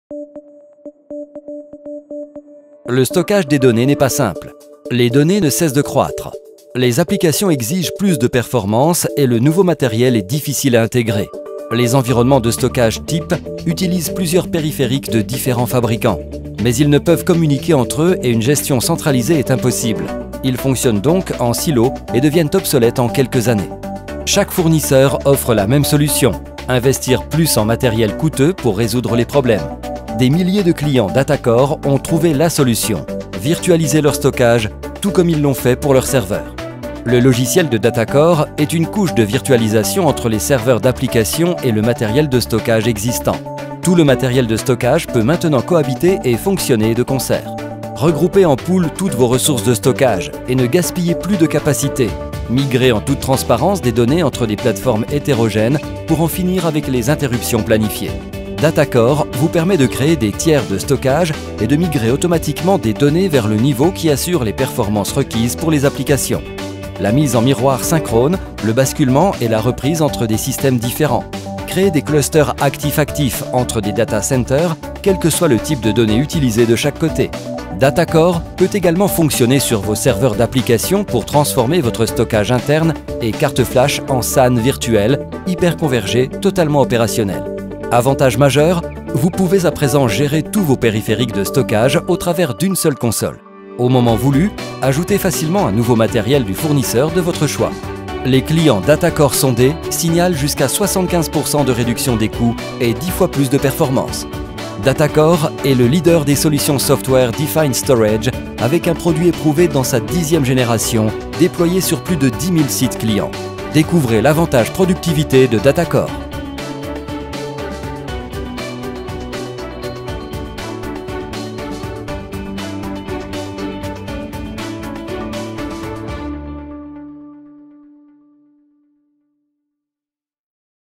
Le logiciel de virtualisation du stockage de DataCore (Français)
SANsymphony™ est notre solution phare de virtualisation du stockage, dans sa 10e génération. Déployée sur plus de 10000 sites clients, elle optimise l'efficacité, la disponibilité et l'utilisation des infrastructures informatiques, en virtualisant le matériel de stockage.